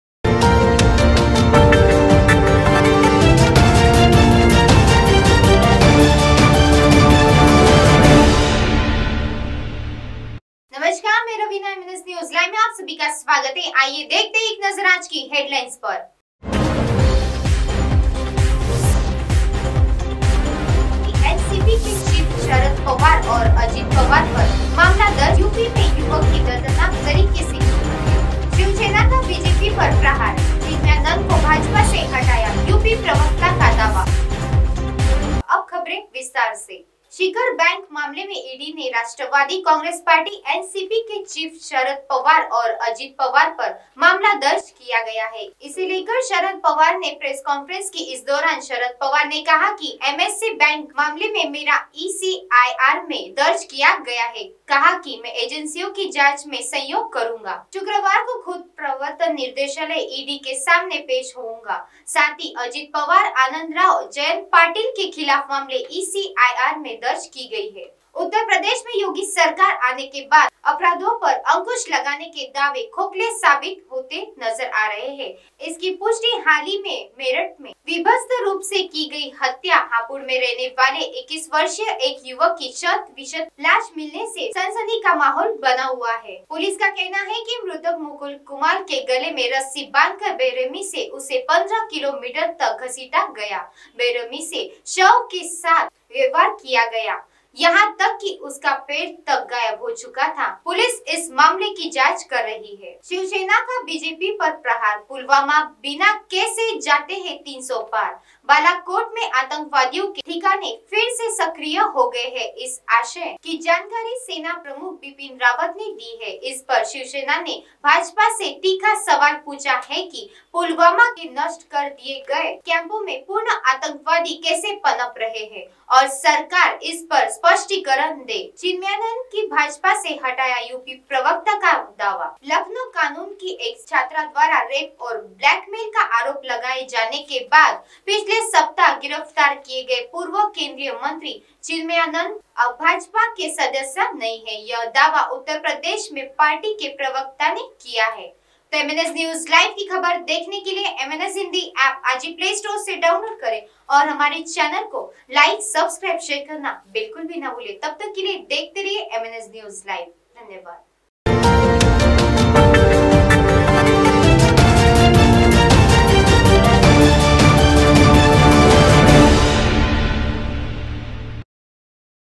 के चीफ शरद पवार और अजित पवार पर मामला दर्ज।
MNSNEWSLIVE NCP के चीफ शरद पवार और अजित पवार पर मामला दर्ज।